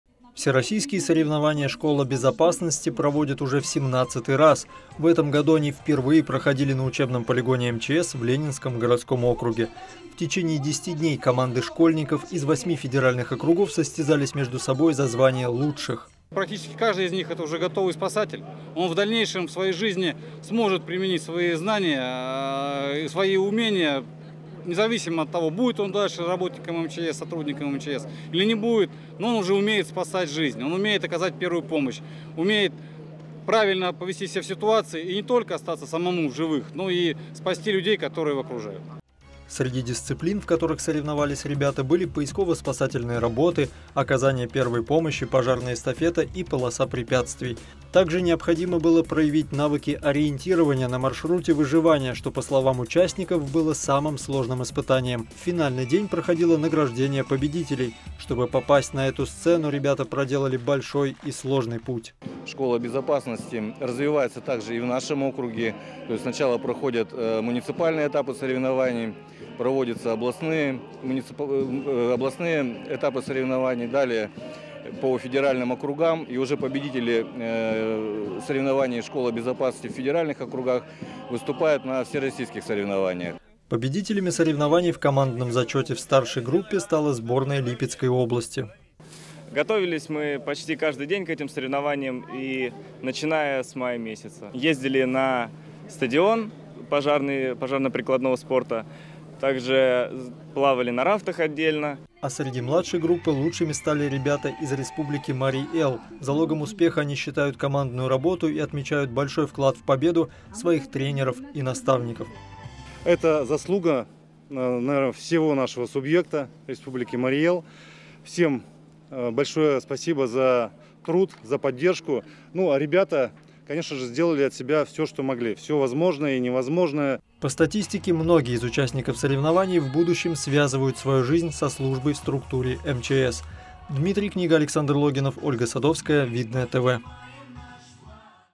ШКОЛА БЕЗОПАСНОСТИ
С 1 по 10 августа на учебном полигоне МЧС в деревне Апаринки проводились соревнования среди школьников со всей России «Школа безопасности». Все эти дни ребята жили в полевых условиях и проходили конкурсные испытания. В последний день мероприятия состоялось торжественное награждение победителей